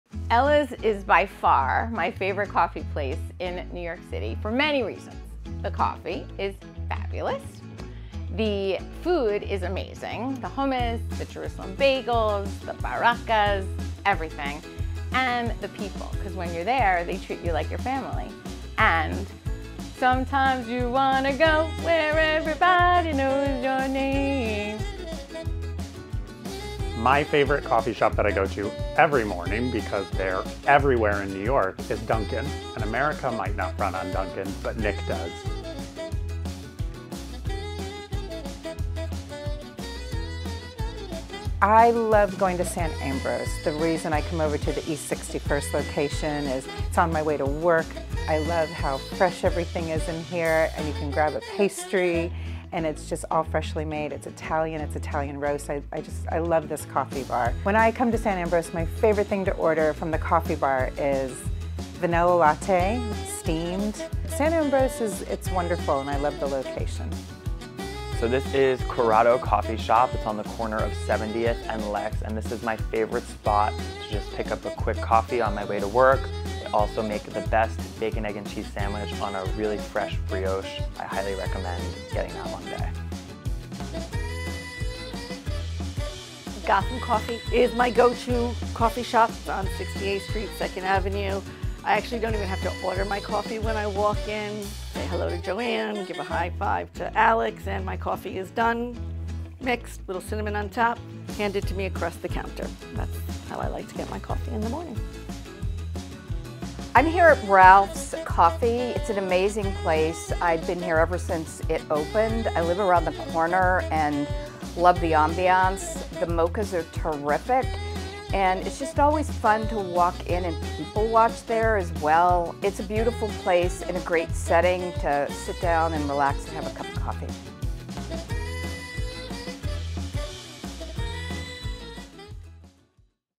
Here is our list of the 6 Best Coffee Shops in the Upper East Side ❤️
In honor of Valentine's Day, one thing our entire team LOVES is coffee! ☕❣️

Our list of favs includes Ellas Cafe, to Dunkin, to Sant Ambroeus, to Corrado Cafe, to Gotham Roasters, and Ralph's Coffee.

What is your favorite spot in New York City to grab a cup of Joe?